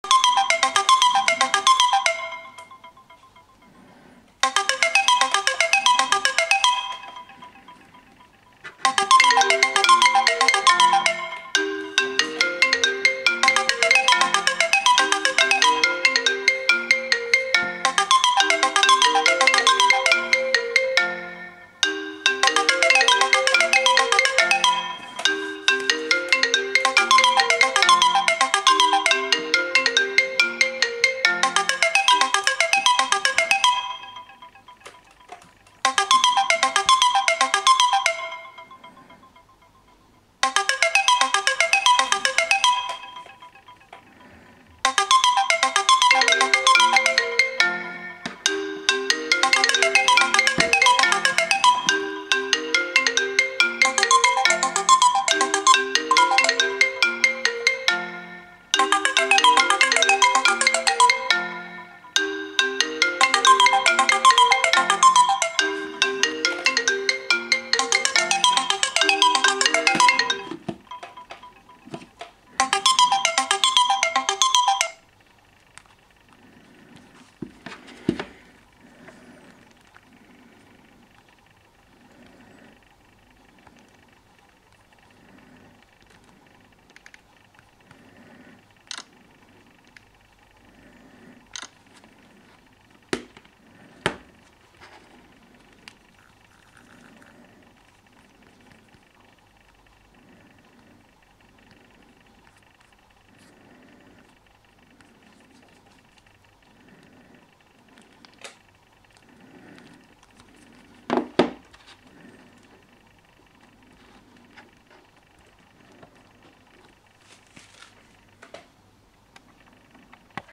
incoming call & Outgoing call & alarms at the Same Time Iphone 7 plus+htc+Samsung S4 mini android 11
incoming call & Outgoing call at the Same Time  Iphone 7 plus+htc+Samsung Galaxy S4 mini android 11